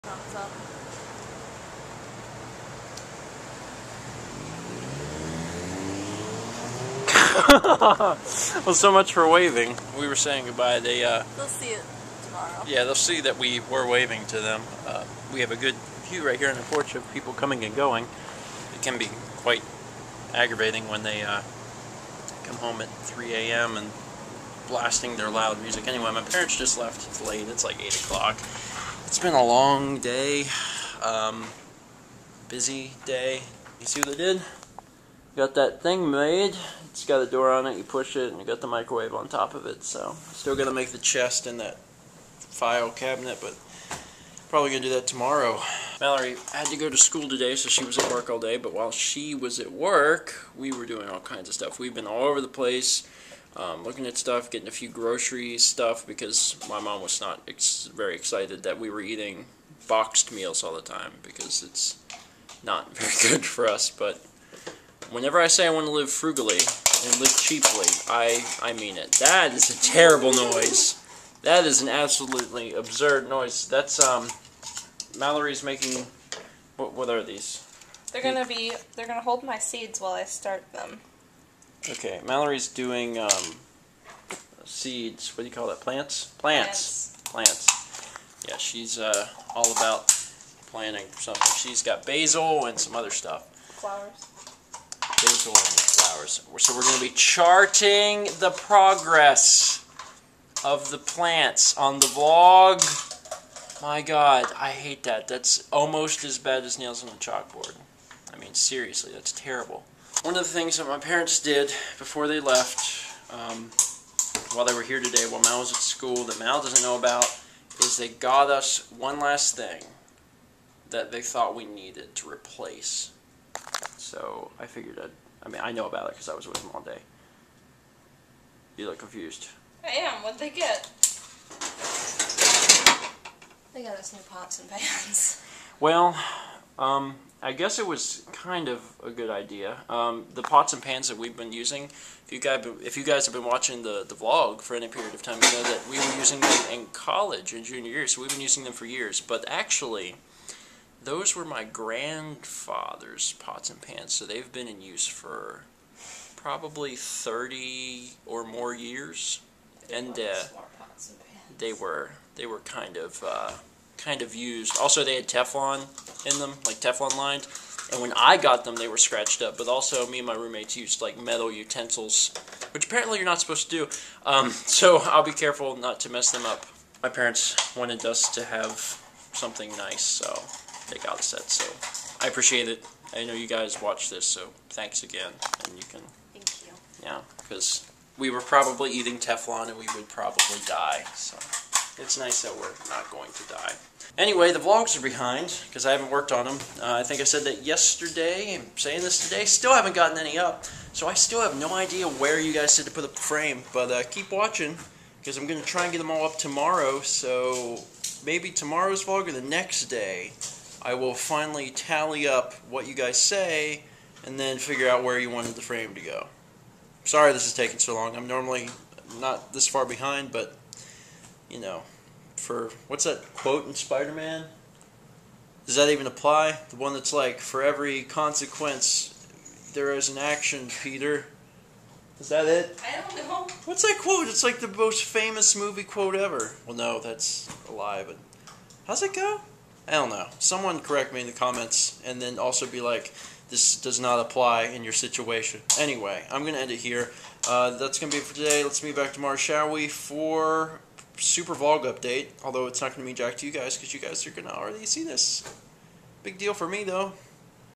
THAT is a TERRIBLE NOISE! (Day 636 - 8/22/11)
My parents head back home, and I review the state of the apartment, including surprising Mal with the last thing my parents bought us.